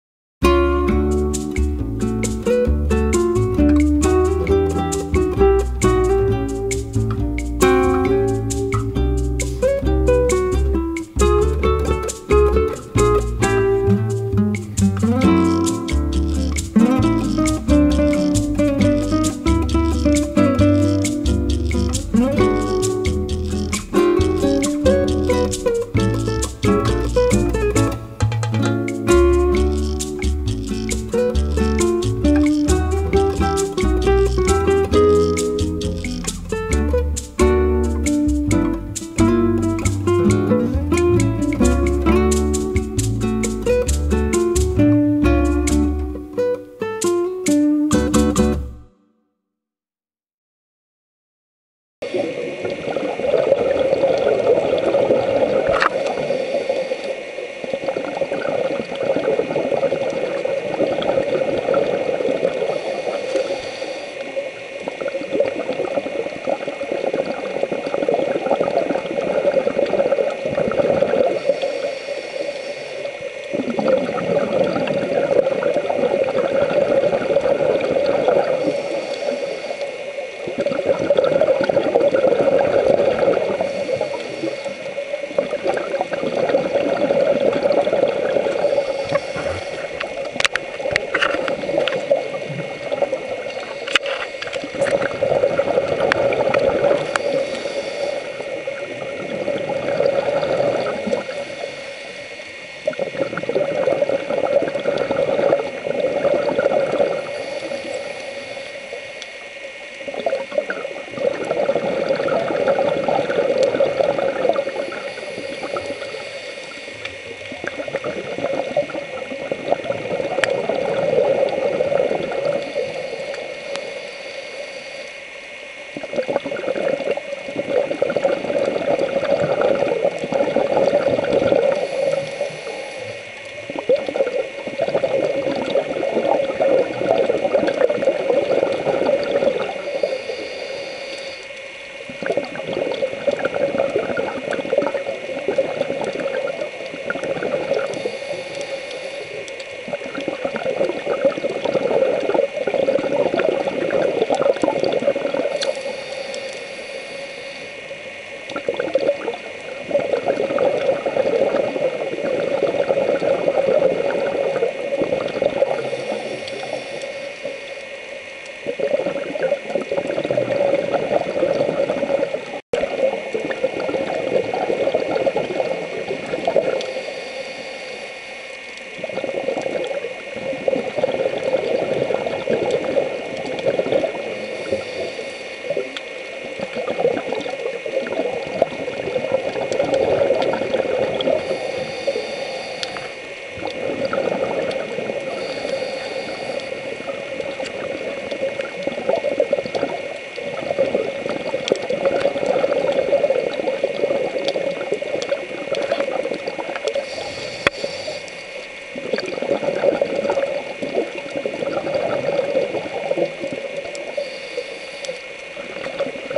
Pelican Island dive.wmv
1st solo dive for Karen and Sandy